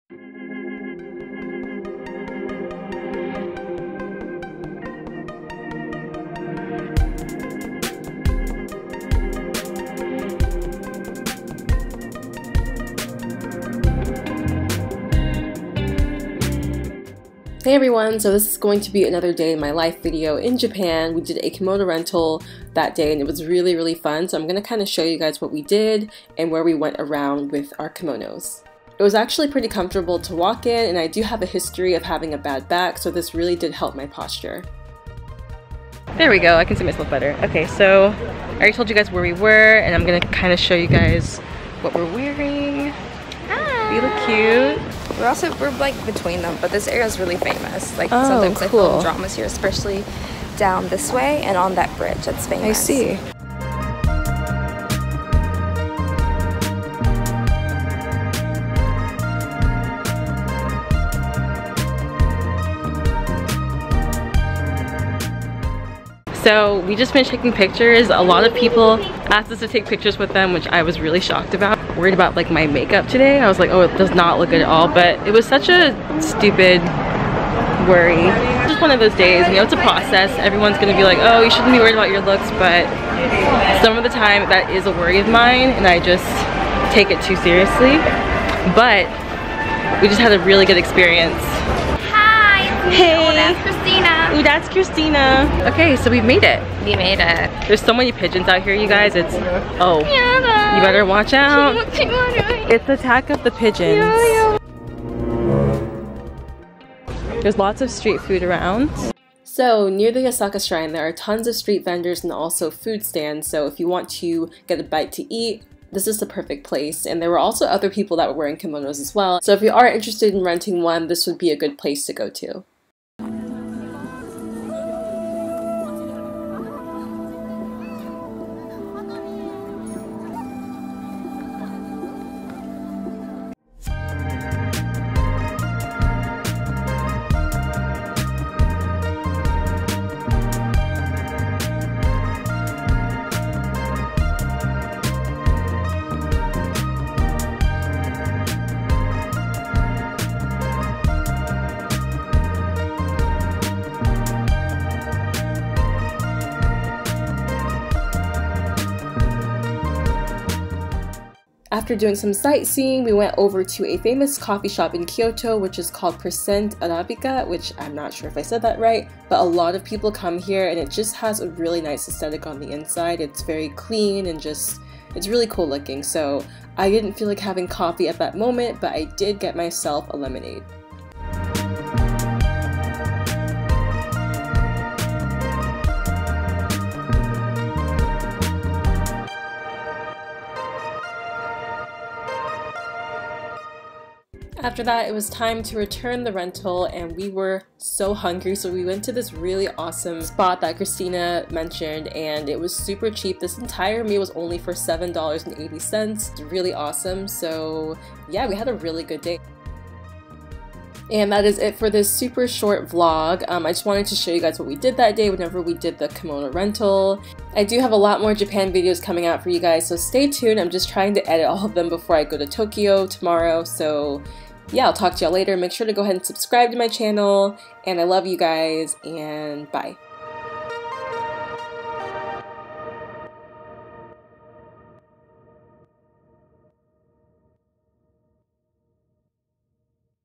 I rented a Kimono for a day in Japan! | Timaloveslemons
More videos coming soon from Japan!!
Hey! I'm Tima Love, and I make beauty/lifestyle videos on youtube!
-Tima